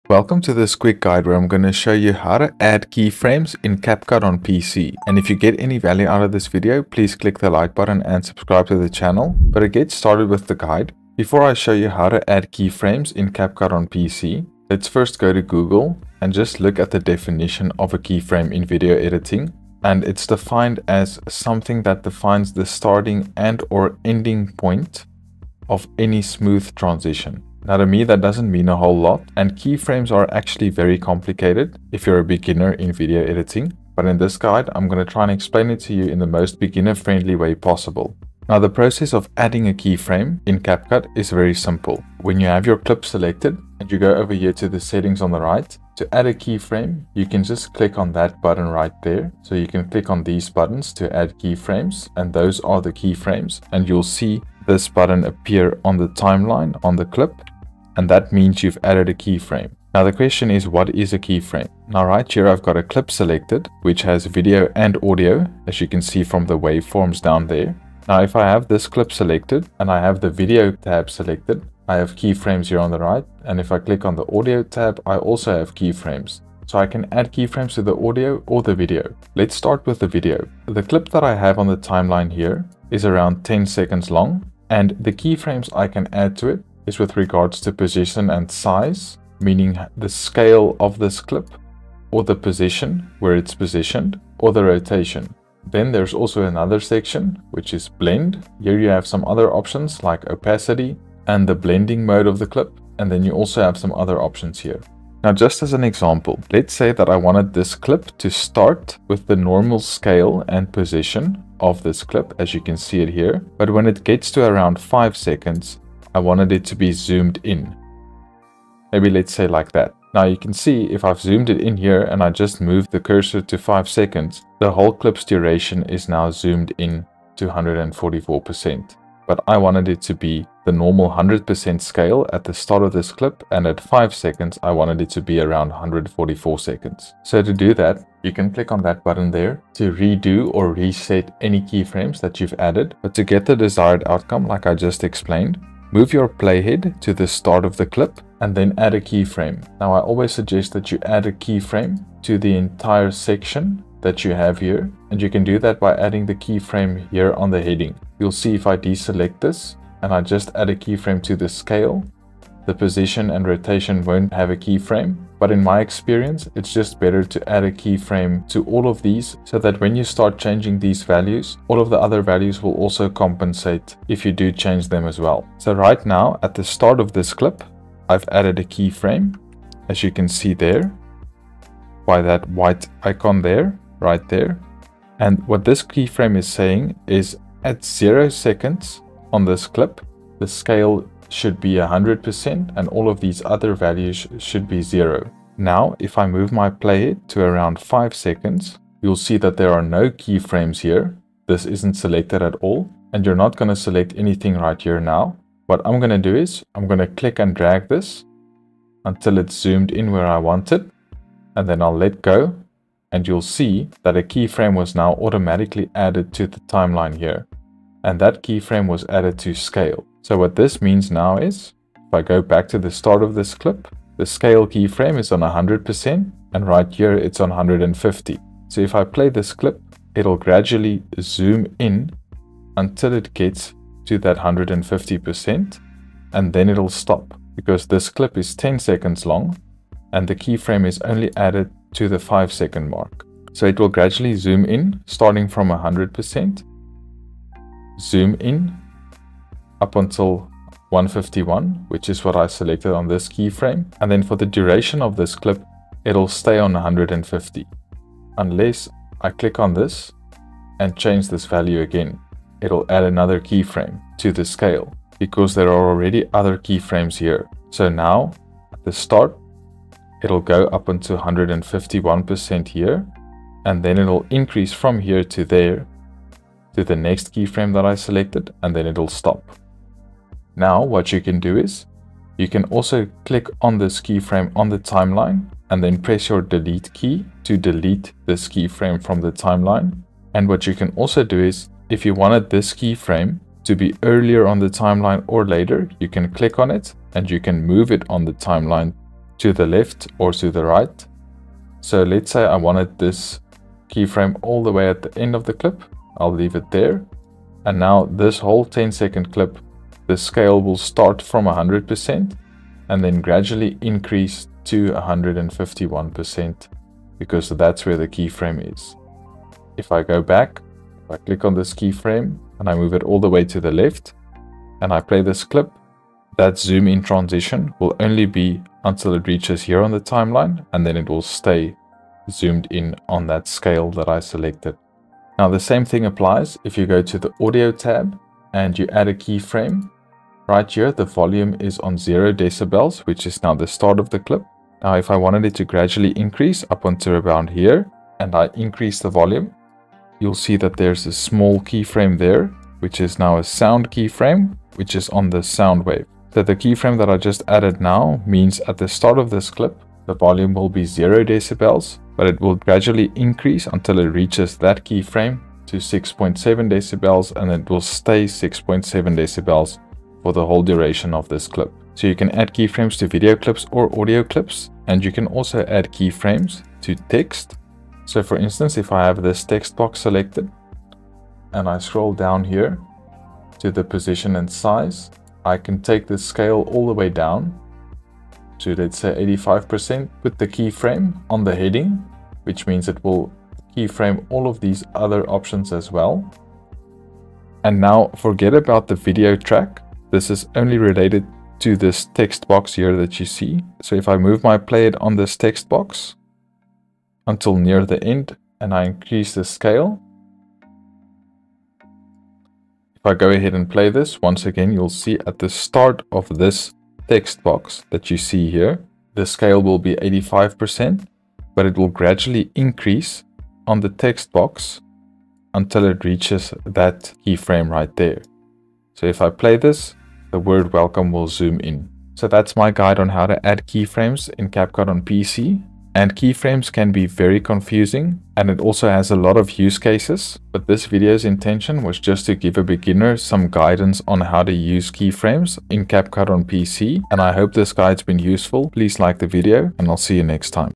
Beginners Guide on How to Add Keyframes in CapCut PC (Tutorial)
In this tutorial video, I'll show you how to add keyframes in CapCut pc if you're a complete beginner. So, if you're new to video editing and keyframes and you've been wondering how to add keyframes to text, video or audio in CapCut pc, this video is for you as I'll take you through the entire process step-by-step.